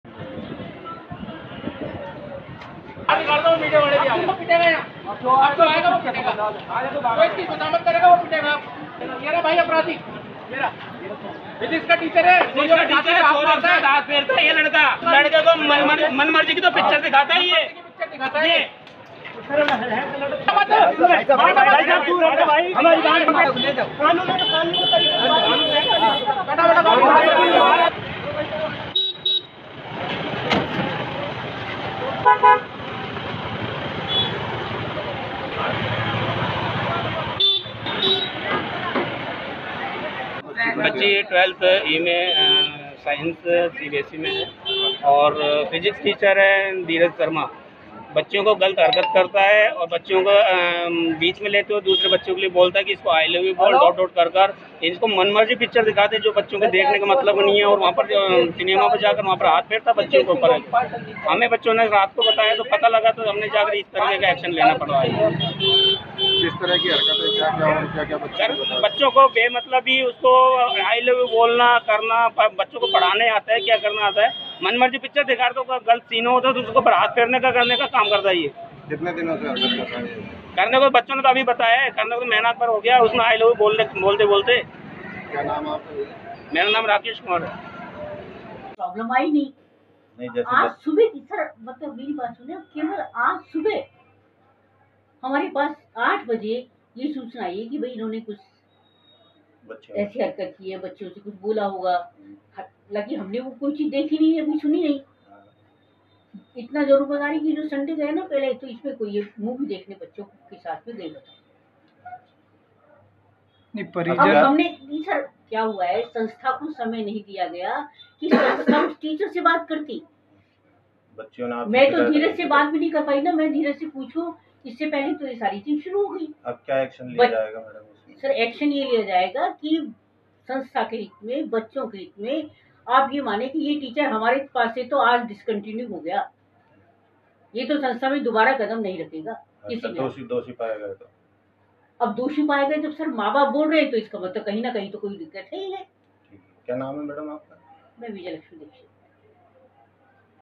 शिक्षा का मंदिर हुआ शर्मसार, टीचर ने स्टूडेंट को दिखाई अश्लील फिल्म
अलवर। शिक्षा का मंदिर एक बार फिर से शर्मसार हुआ है. एक कोचिंग के टीचर के द्वारा एक स्टूडेंट से छेड़छाड़ और उसको अश्लील फिल्म दिखाने का मामला सामने आया है.